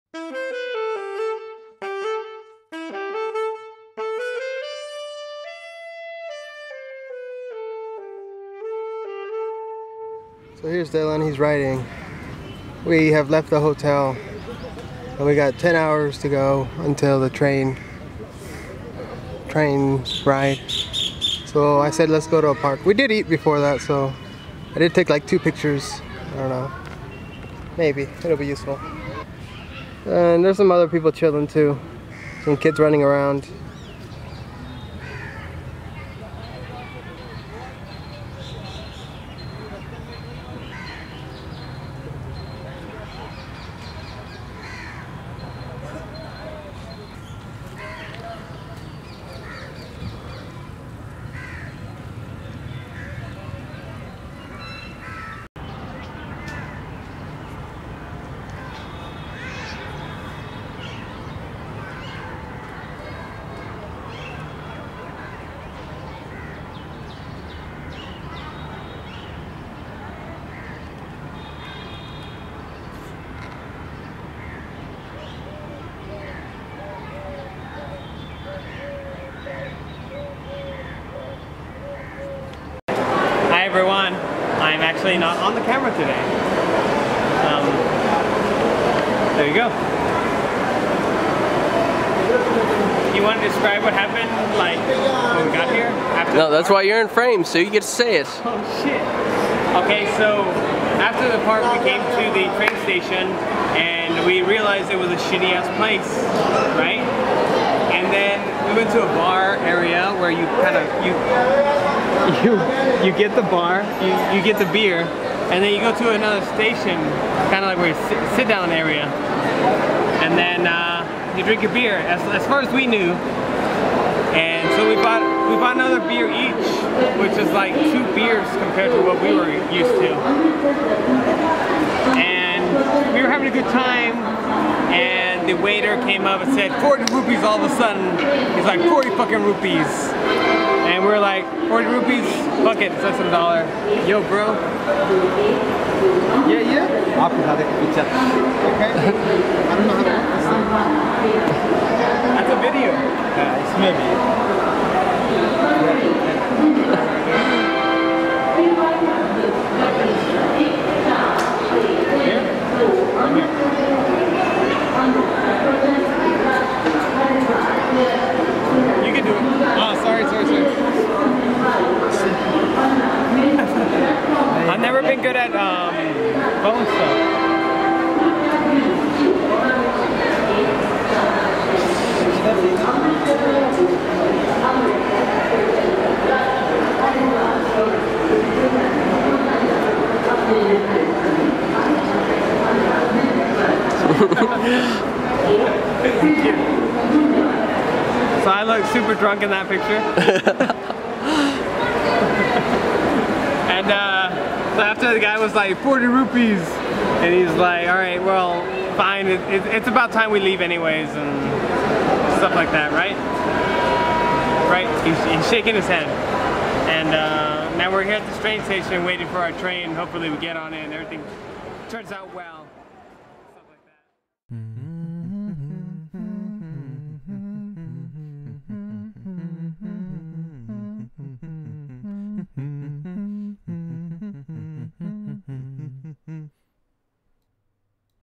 [Day 15] - meh
We didn't do much this day. Just waiting on our train.